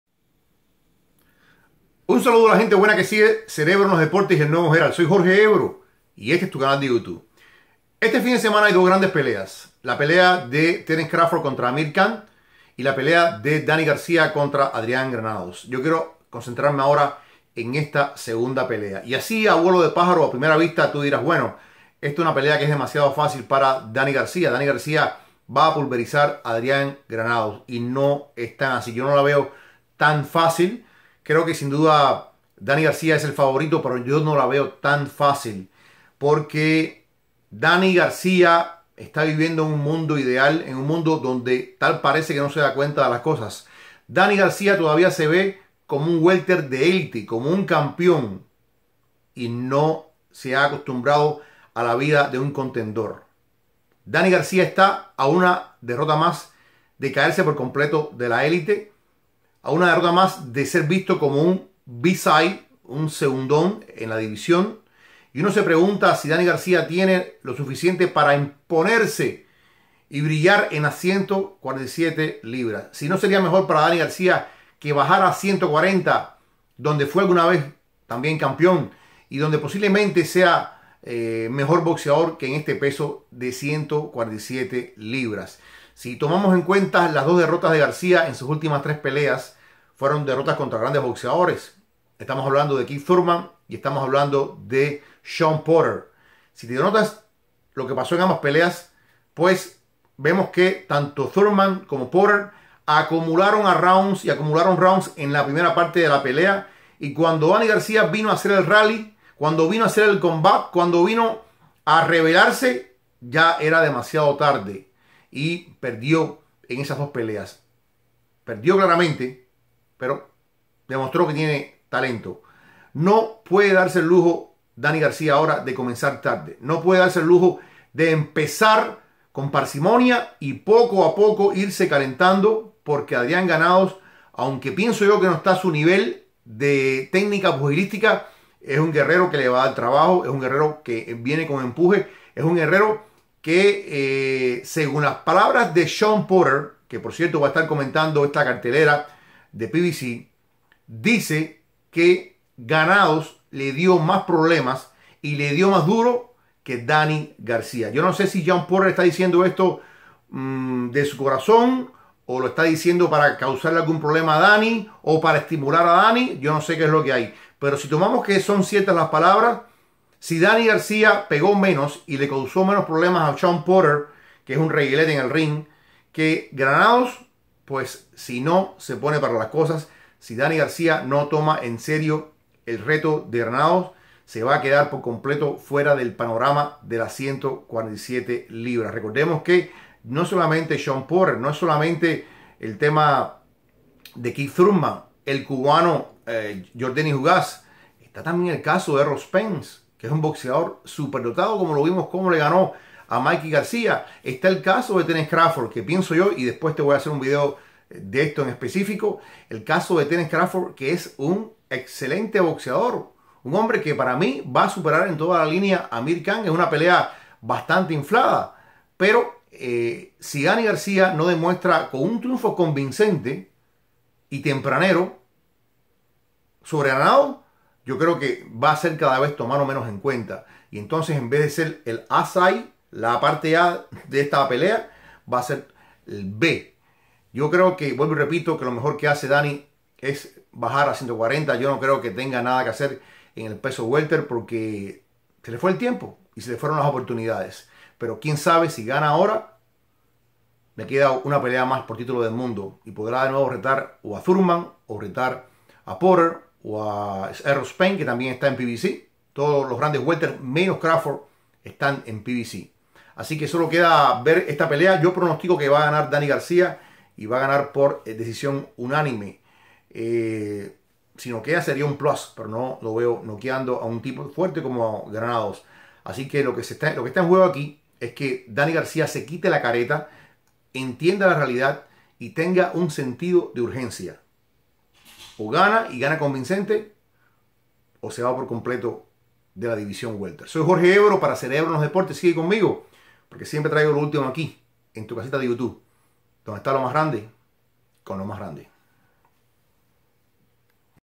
Danny García debe impresionar o bajar
Danny García debe mostrar que sigue siendo un guerrero de élite en las 147 libras, después de dos derrotas ante Keith Thurman y Shawn Porter. Granados no está  a su nivel, pero se entrega con fuerza y le será difícil. Tal vez García deberia bajar a las 140 libras.